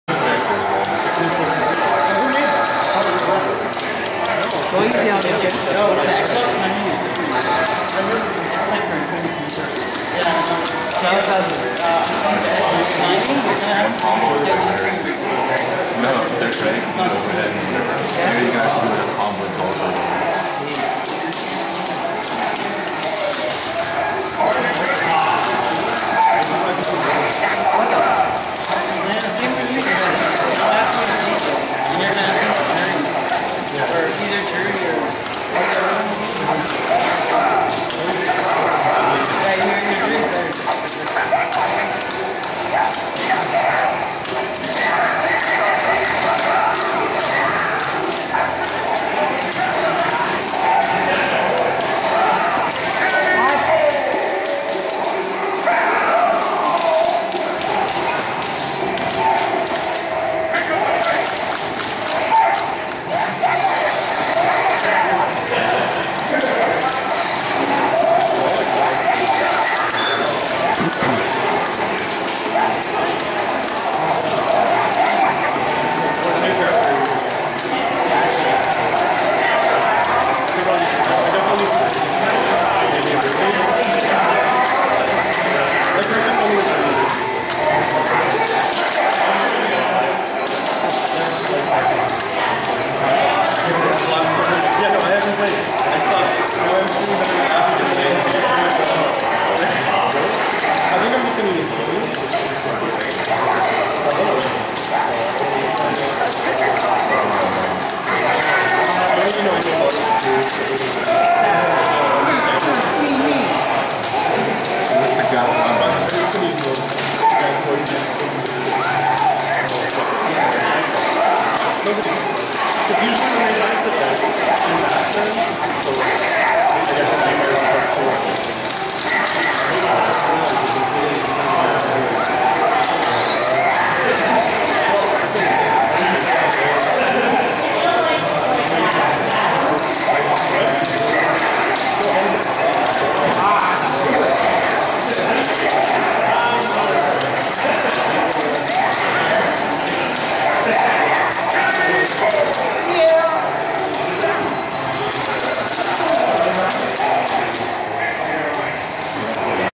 TFNS Toronto Fight Night Pratice 3
Adon VS Dudley
Pratice fights May 01 2010